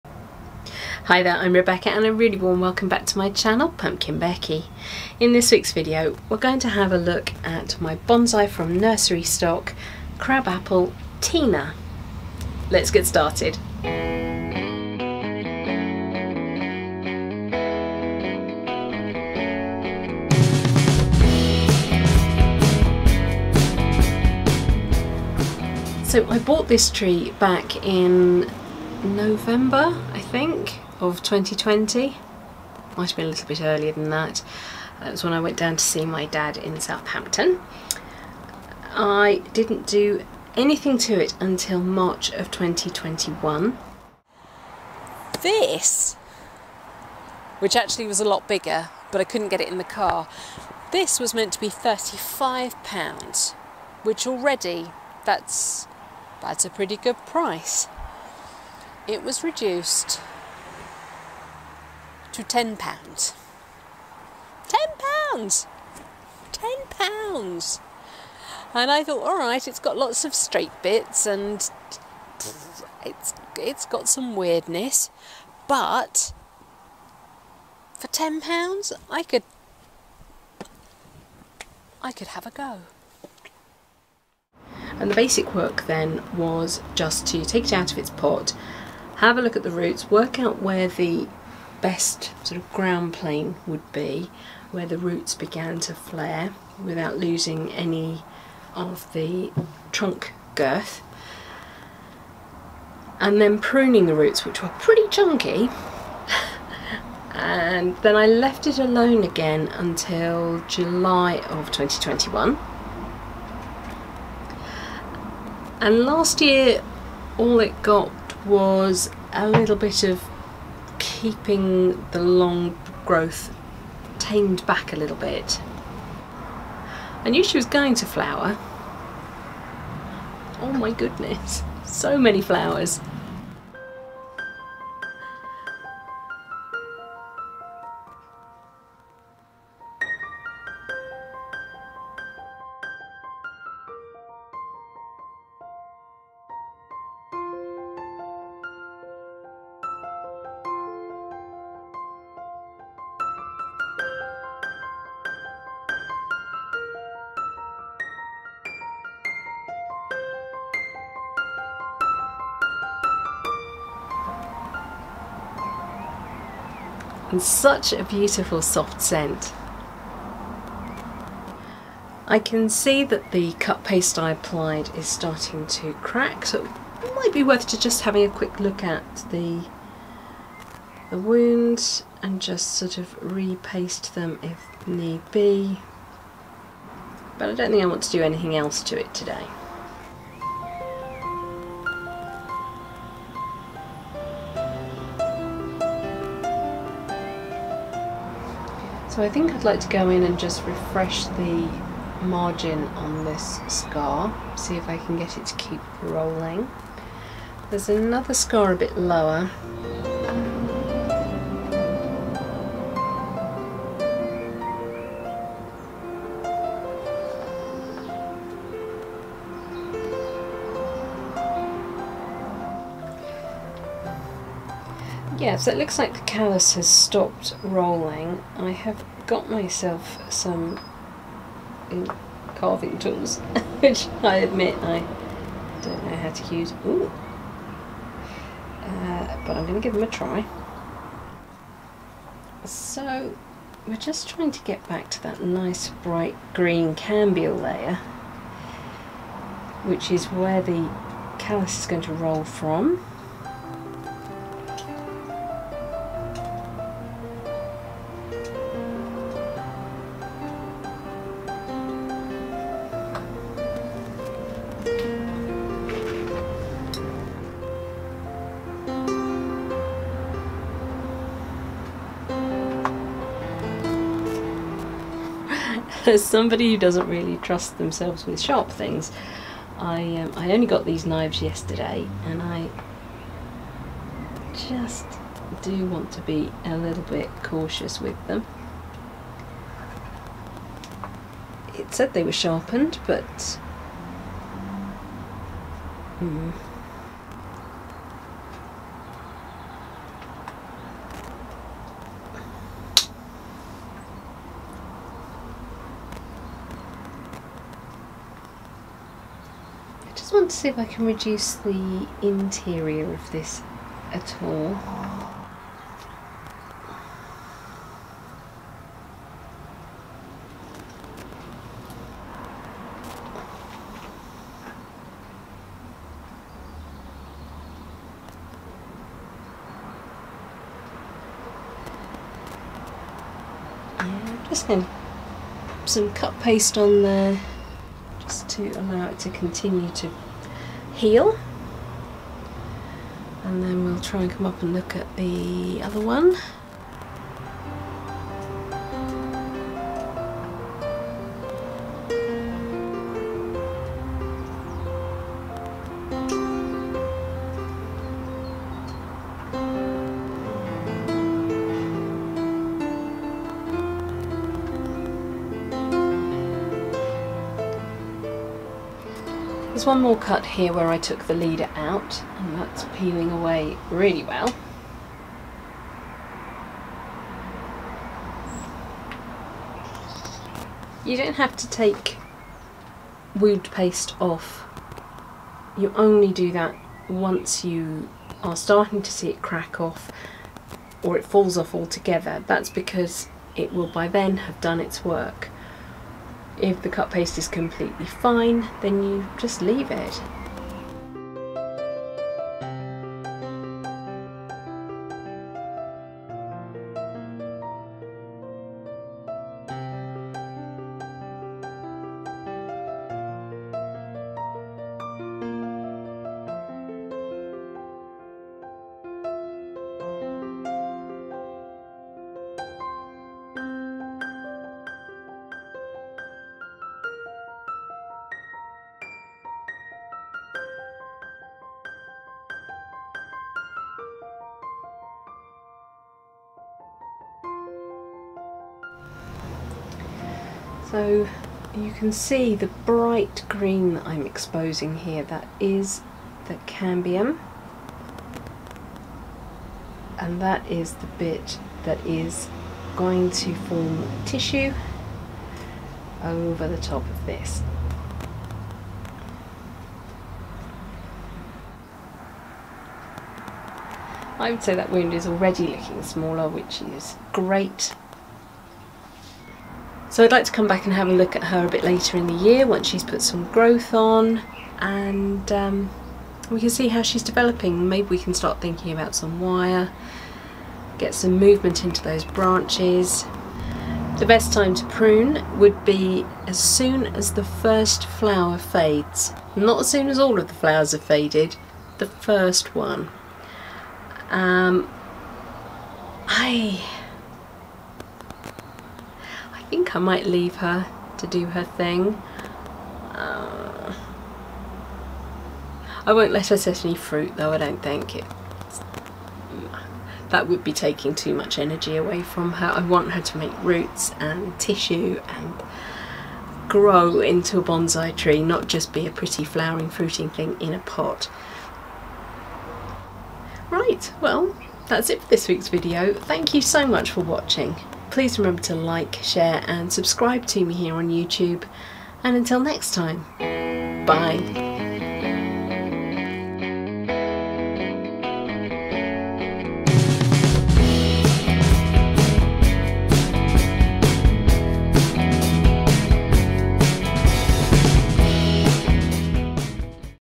Flowering Bonsai - Crab Apple Tina update - May 23
Hi everyone,
You may have seen a Community post about my Crab Apple "Tina", but I am so blown away by the quantity of flower on this pre Bonsai that I just had to film a video too. We'll also take a look at how the the cuts are healing where I made big fundamental branch reductions, do a bit of carving, and apply more cut paste.
Love
Rebecca xx
Instagram: @pumpkinbecki and tag me with pumpkinbecki